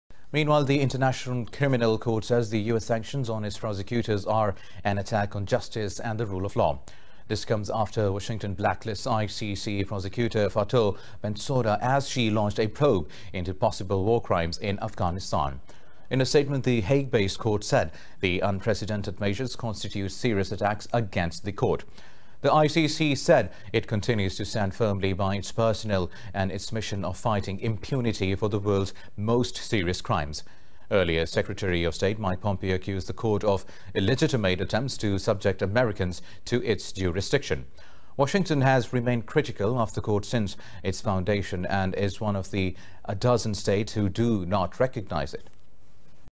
ICC: U.S. Curbs Infringe International Rule Of Law | Indus News | Indus News Bulletin
The International Criminal Court said the United States sanctions on its prosecutor are an attack on justice and the rule of law.